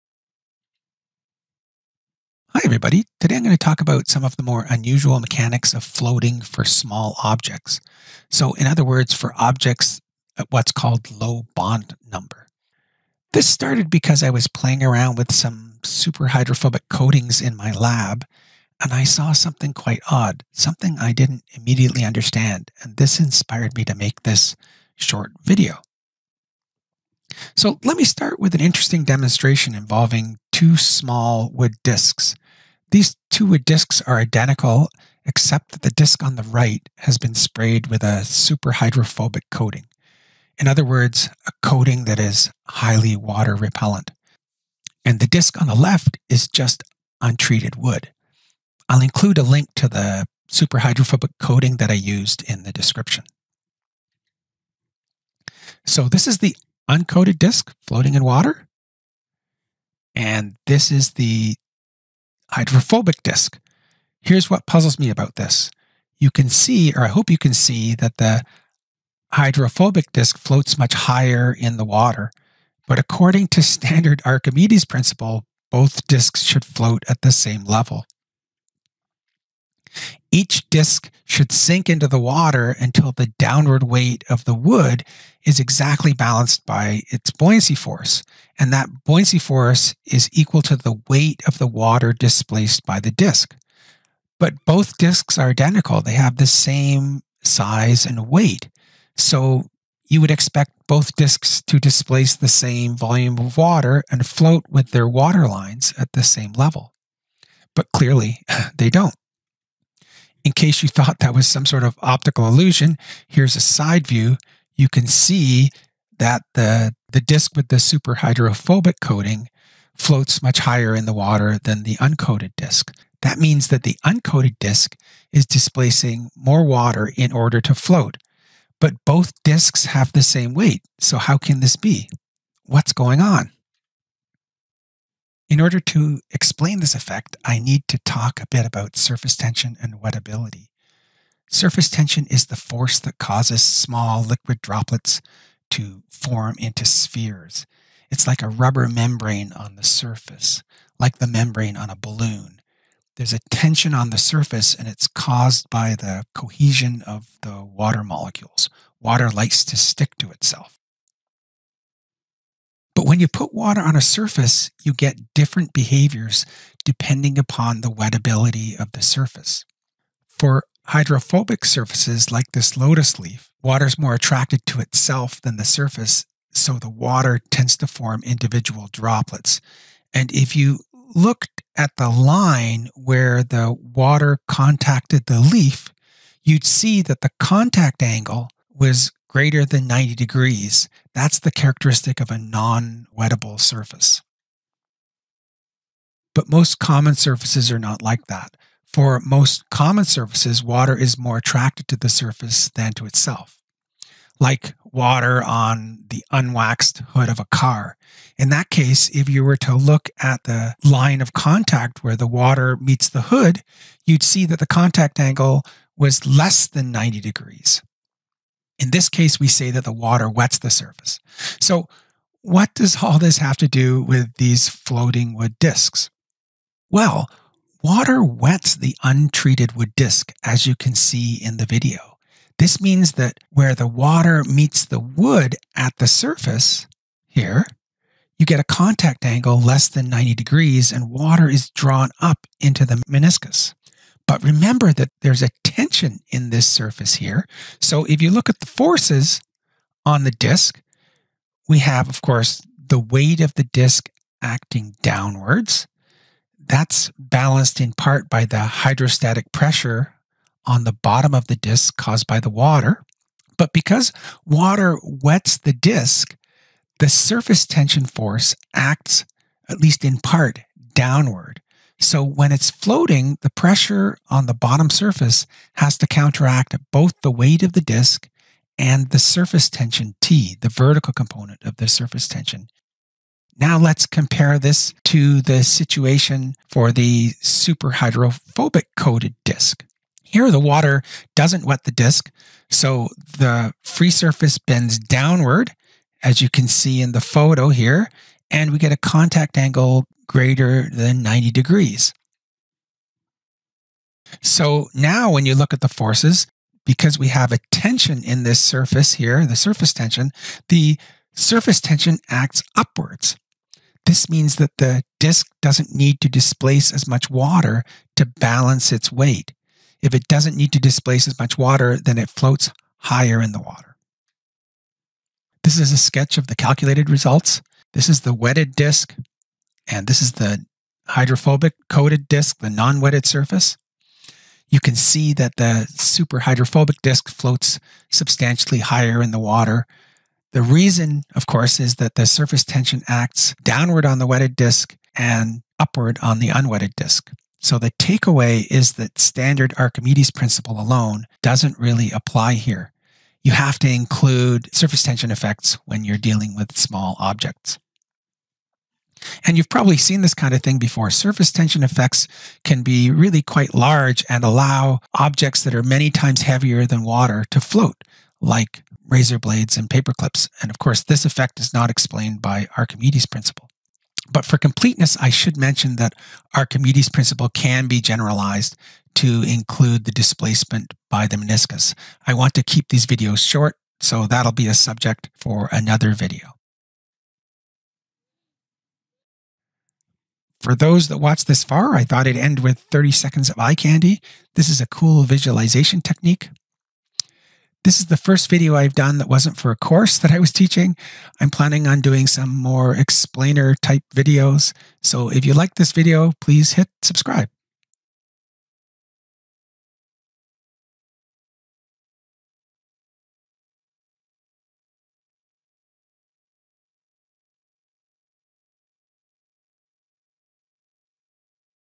The Role of Surface Tension in Floating
This video explains some of the mechanics of floating for small objects (low Bond number). Surface tension and wettability effects become important for small objects. As a result, an object with a water repellent (hydrophobic) coatings will float higher in the water column than the same object with a wettable surface.

In this demonstration, the wood disks are 10 mm in length and 18 mm in diameter, floating in water. The coating is a 2-step super-hydrophobic commercial coating: Rustoleum Neverwet.

Nowadays, Archimedes’ principle can be generalized to include weight of fluid displaced by the meniscus. I hope to discuss this in a separate video. You can find the mathematical proof of the general equivalency that the weight of the fluid displaced by the meniscus is equal to the surface tension force here:

Keller, J.B., "Surface tension on partly submerged body", Physics of Fluids, vol. 10, no. 11, 1998.

This journal paper (by the Author) provides more information on the mathematical solution and the demonstration setup: